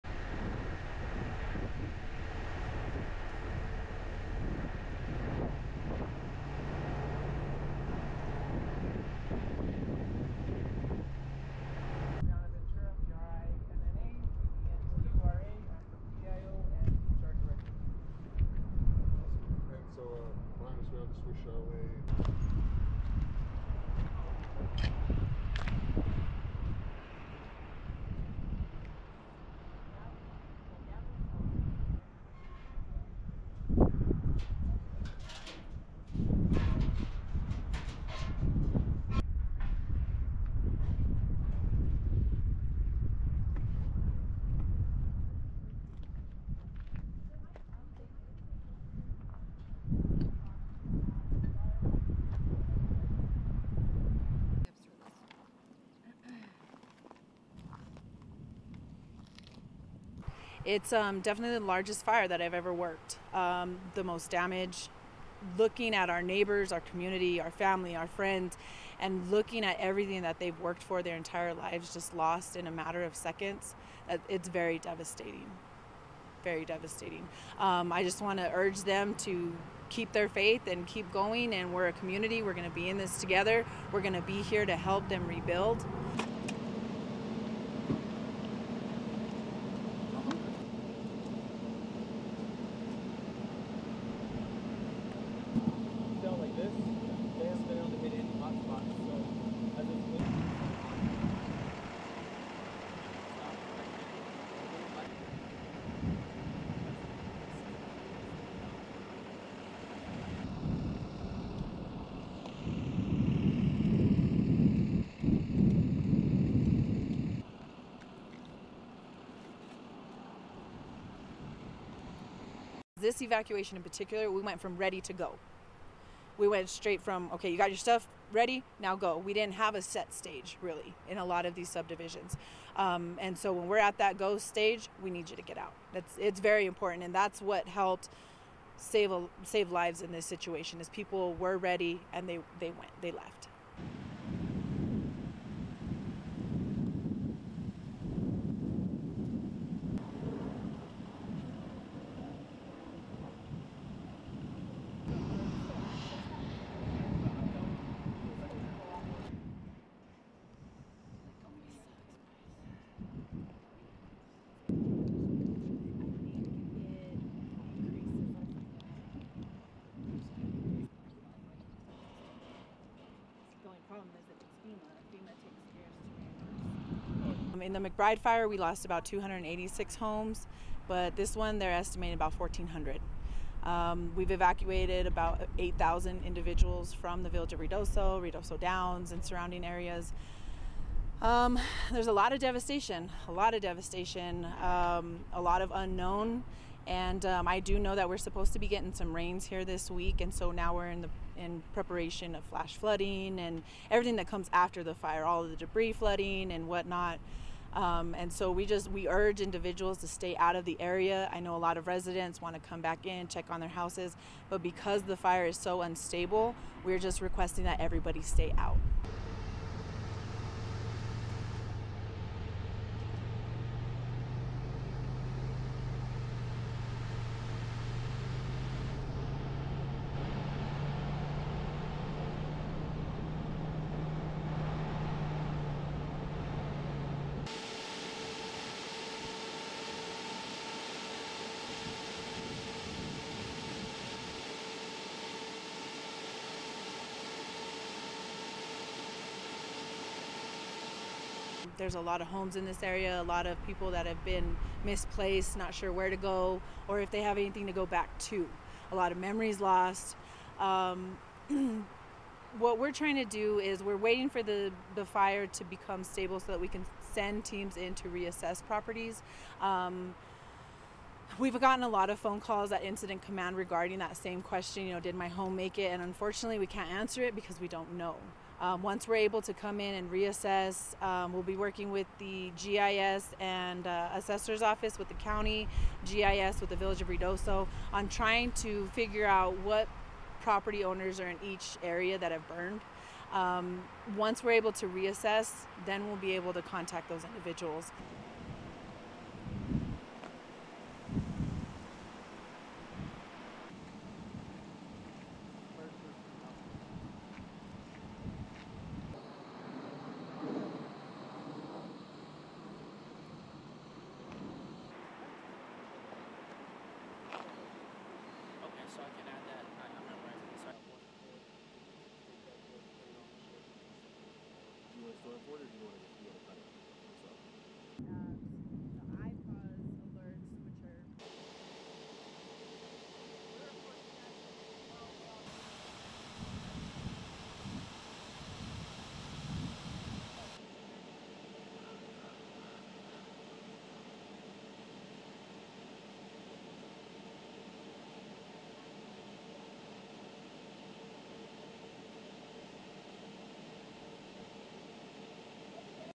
'It's very devastating': Apocalyptic scene as South Fork Fire ravages town of Ruidoso | ABS-CBN News
It was an apocalyptic scene on Wednesday in the town of Ruidoso, New Mexico, after the active South Fork Fire swept through the community, causing mass devastation, and flattening many homes.